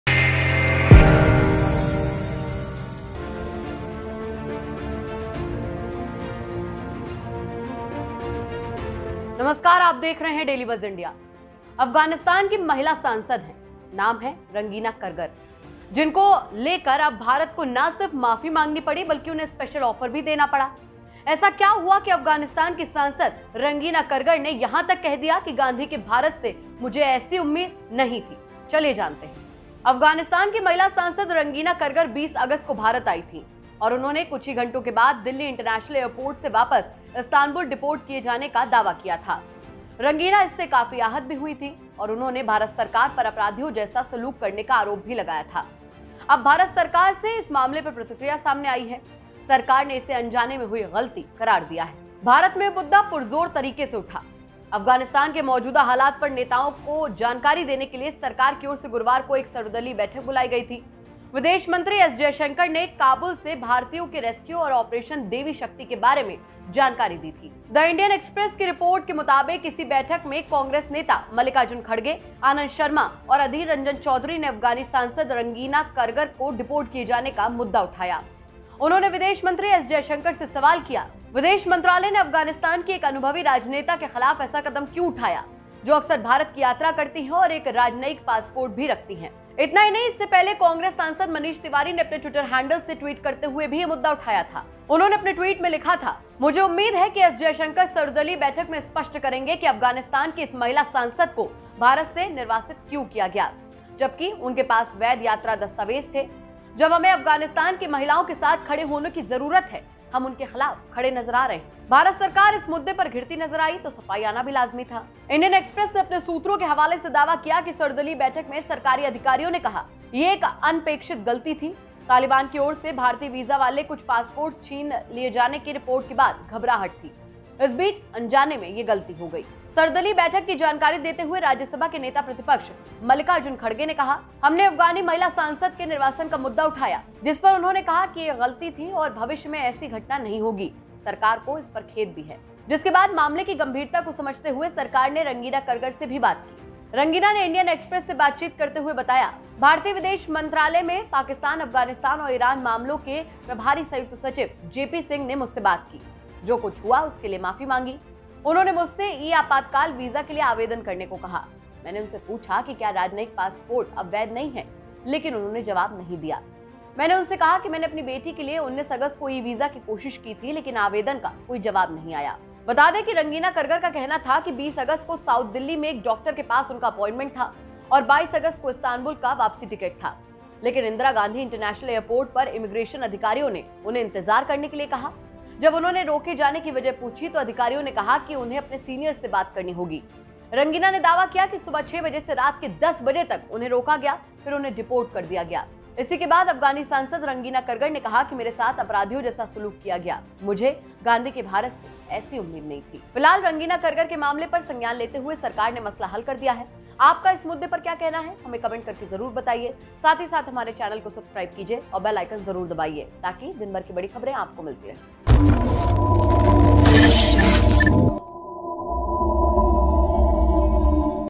Afghan MP Rangina Kargar ने पूछा क्या सिर्फ हिंदुओं को मिलेगी Entry? India ने दिखाया बड़ा दिल
Afghanistan's female parliamentarian, Rangina Kargar, came to India on August 20 and claimed to have been deported back to Istanbul from Delhi International Airport a few hours later. As the allegations were also made to behave ... now the reaction has come from the Government of India on this matter ... the government has termed it as an unintentional mistake.